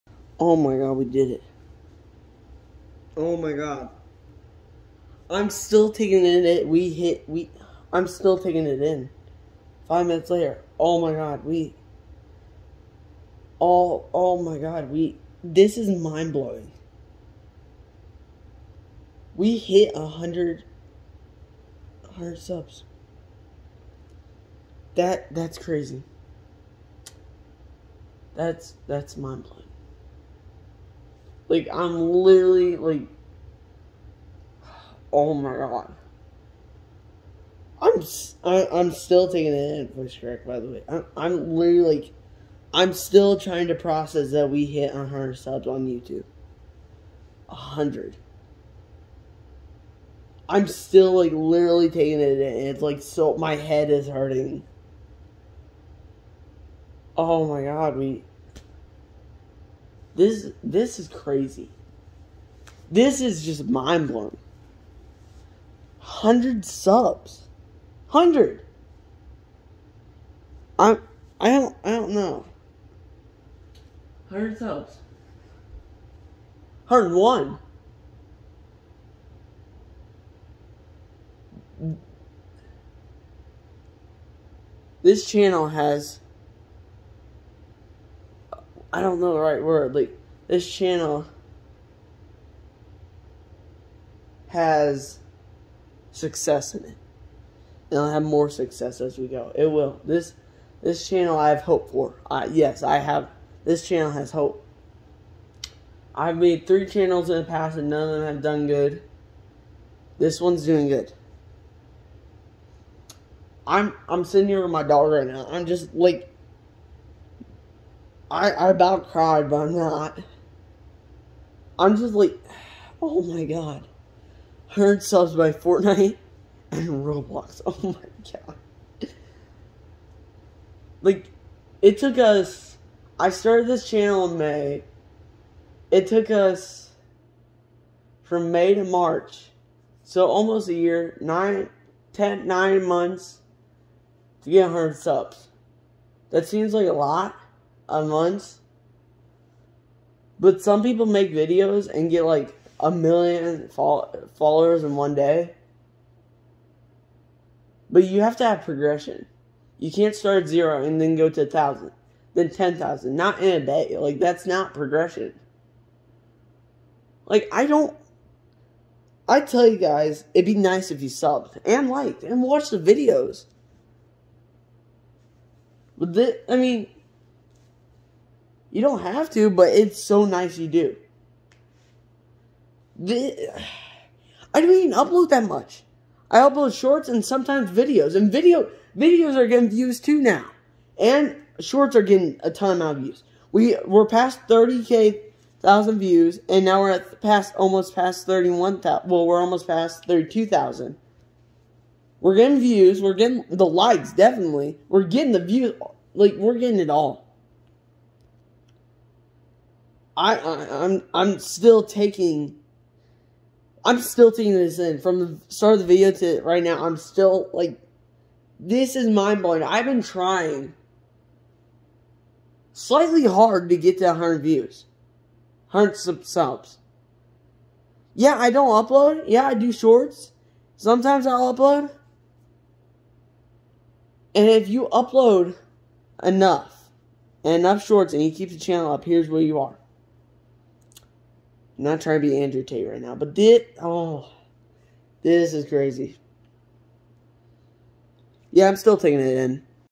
my full reaction to 100 subs
this is best day ever. you don't know how long I've wanted just 100 subs. this is fantastic and this channel has hope.